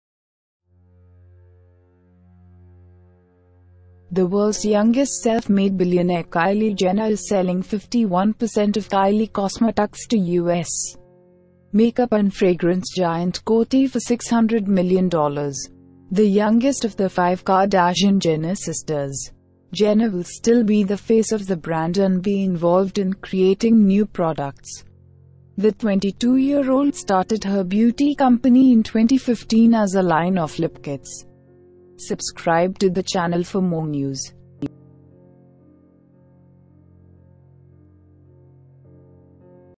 Billionaire Kylie Jenner sells 51% of her beauty company for $600 million
The world's youngest self-made billionaire Kylie Jenner is selling 51% of Kylie Cosmetics to US makeup and fragrance giant Coty for $600 million. The youngest of the five Kardashian-Jenner sisters, Jenner will still be the face of the brand and be involved in creating new products. The 22-year-old started her beauty company in 2015 as a line of lip kits.